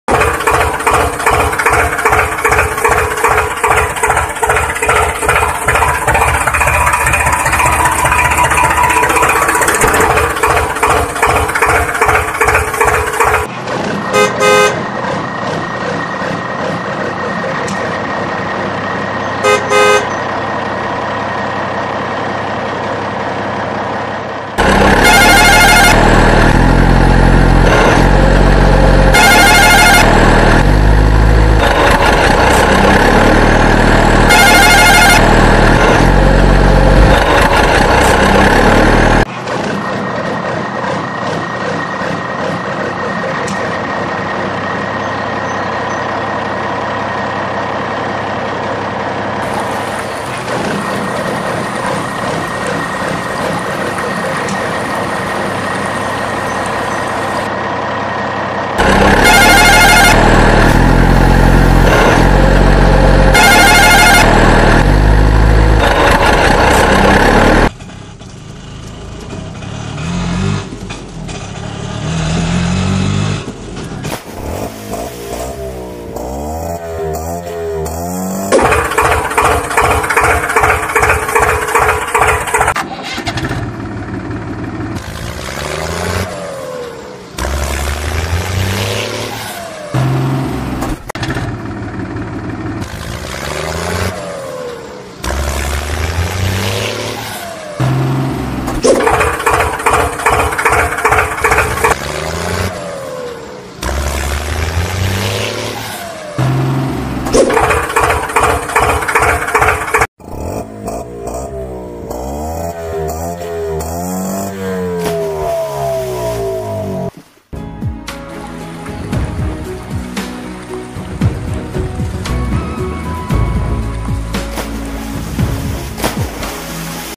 Truck🧸 full of potatoes CNG dumper pulled out Swaraj Mini TractKids Video
Truck full of potatoes CNG dumper pulled out Swaraj Mini Tractor Kids Video @RKTOYS